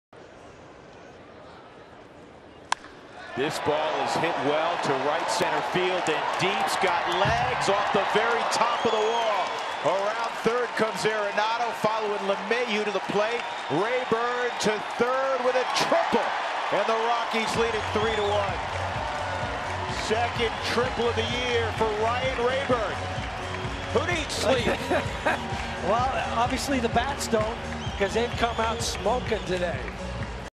PIT@COL: Raburn plates two on a triple to the gap
Ryan Raburn hammers a triple off the wall in right-center, scoring DJ LeMahieu and Nolan Arenado to give the Rockies a 3-1 lead